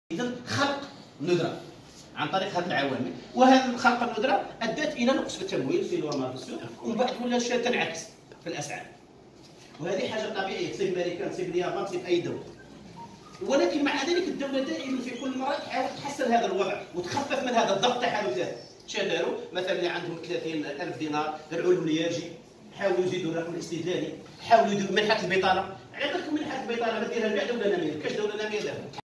يوم اعلامي و تحسيسي بغرفة التجارة و الصناعة ولاية عين تموشنت 28\09\20
يوم إعلامي و تحسيسي الهدف منه توعية المواطنين و المهنيين بمجهودات الدولة لتوفير المواد الاساسية ذات الاستهلاك الواسع من جهة و من جهة أخرى الى تذكير المتعاملين الاقتصاديين بالاحكام القانون ..